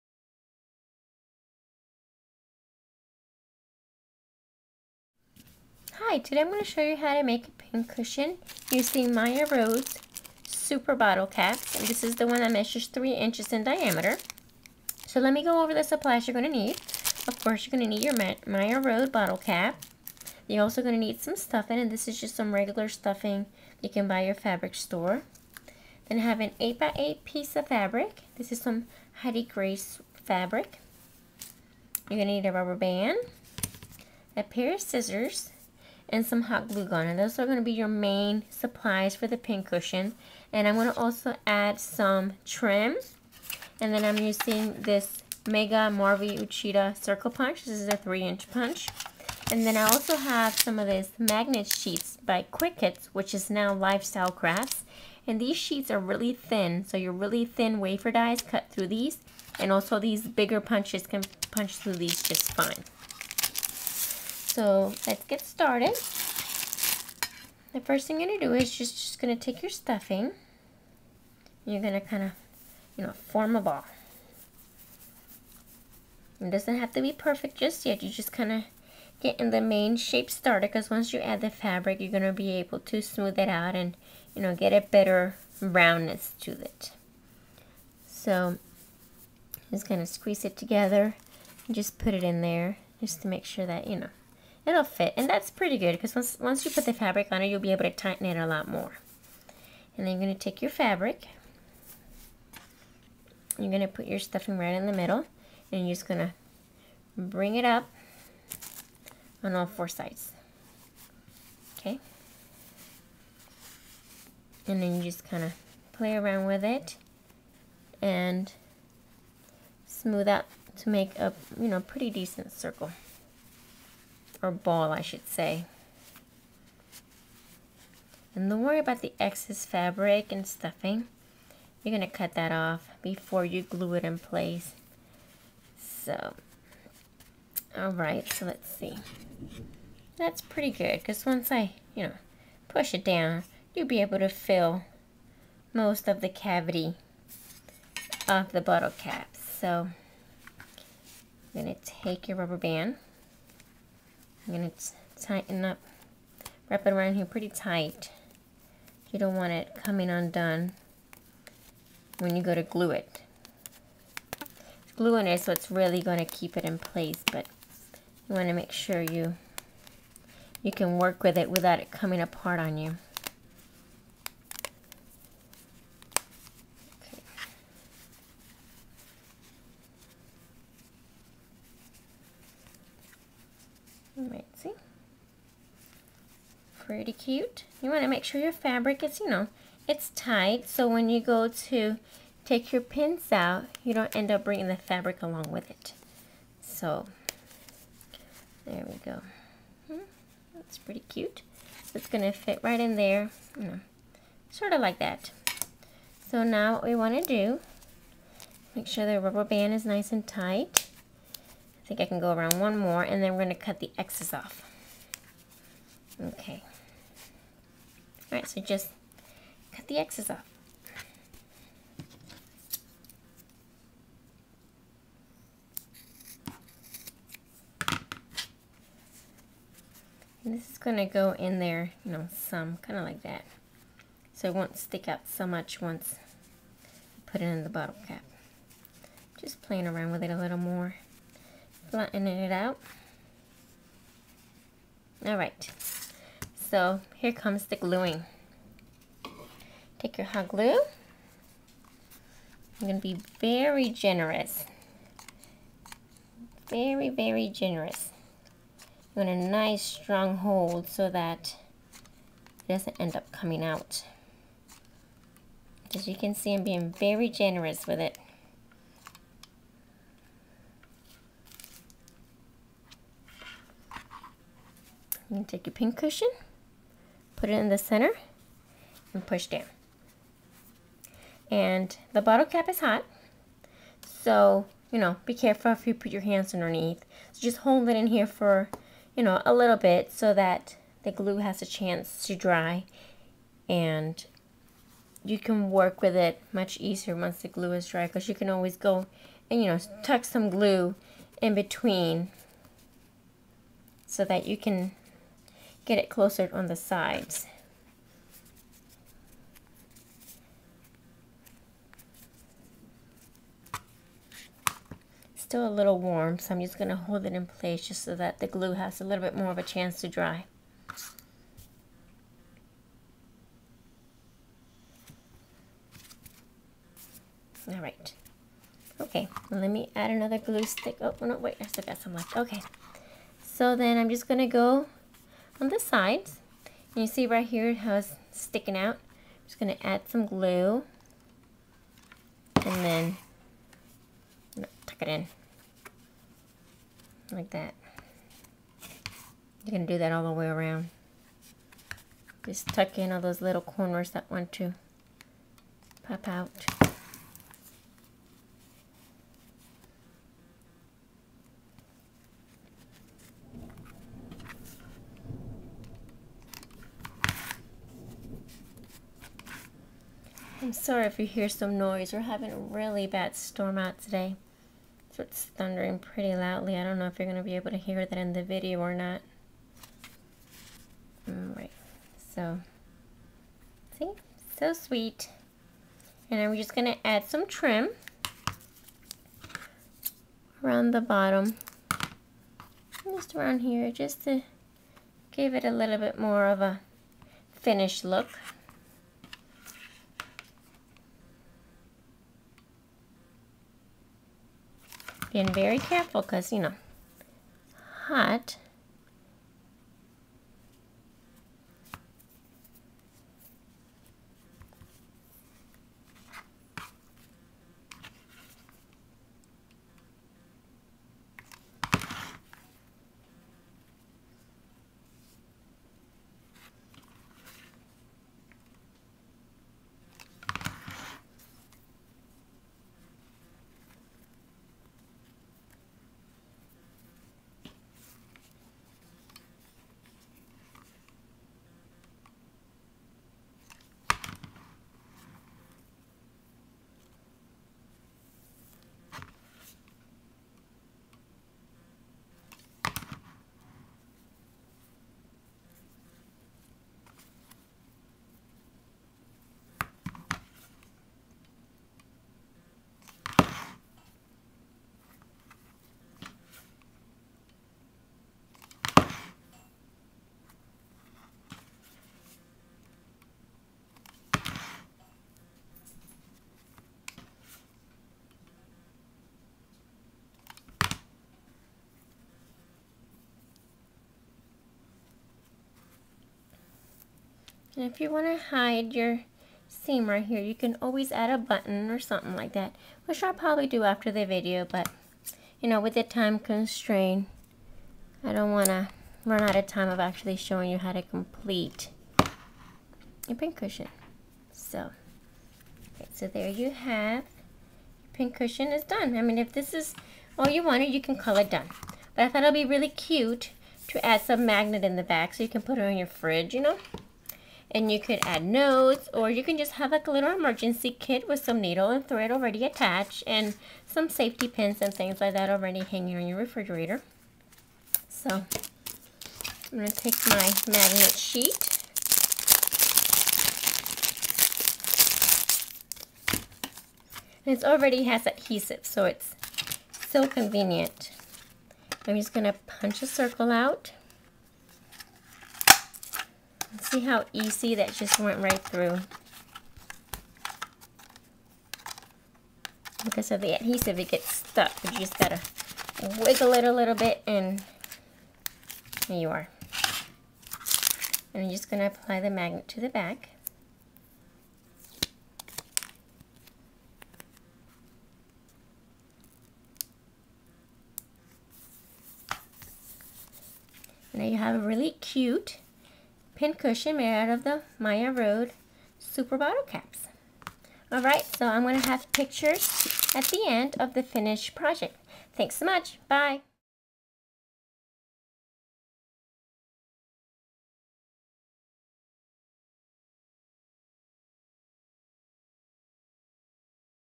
Maya Road Super Bottle Cap Pin Cushion
Creating a PIn Cushion out of Maya Road Super Bottle Cap and Heidi Grace Fabric Small Funky Floral.